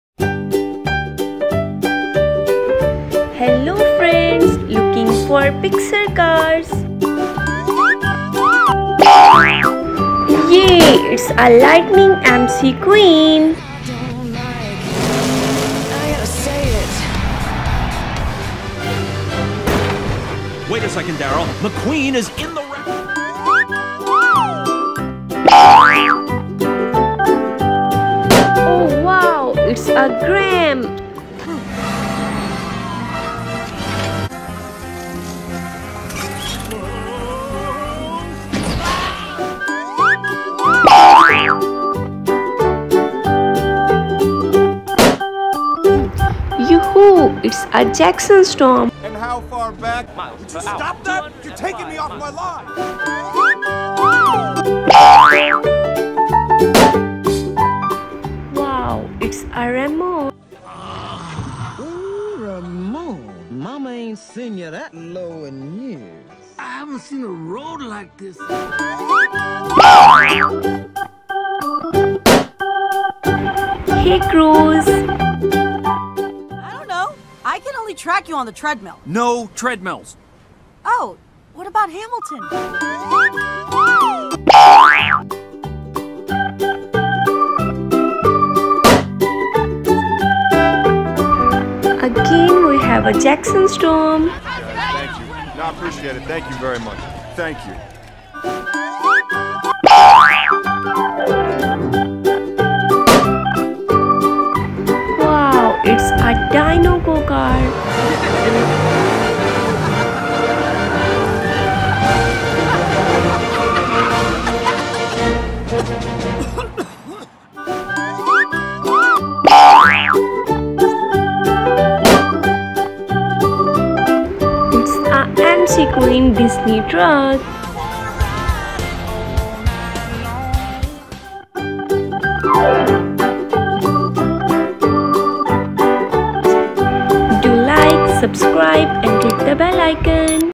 Looking for McQueen DisneyTruck,Lightning McQueen,Jackson storm,Cruz Ramirez blowing up with balloon
Grem , Ramone and many more cars